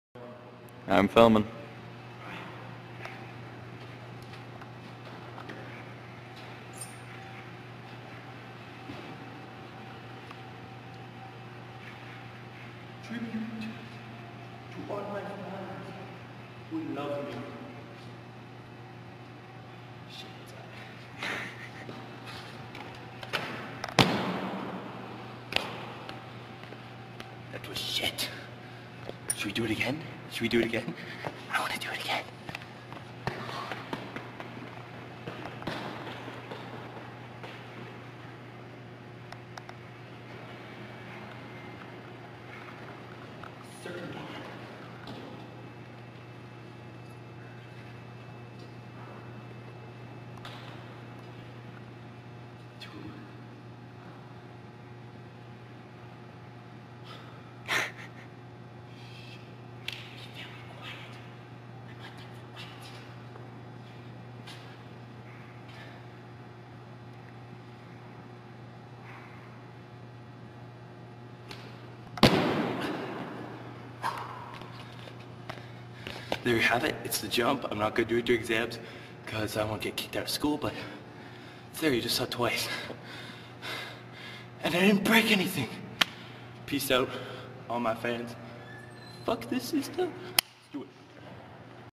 The Infamous 2nd floor Jump
More like a drop, actually.

Well here it is. For all you people who complain they didn't get to see it the first time and constantly tell me to do it again. And it's utter crap anyways. It's so slow and clumsy. Oh well, at least I didn't injure myself.

P.S This is NOT parkour. Parkour isn't just jumping off tall shit, it's vaults and rolls and catleaps, stuff that takes skill. Go practice that instead: it's way more fun, takes more skill and time to perfect, and isn't nearly as dangerous. This video is here simply to get people off my back who keep telling me I should do it again.

Peace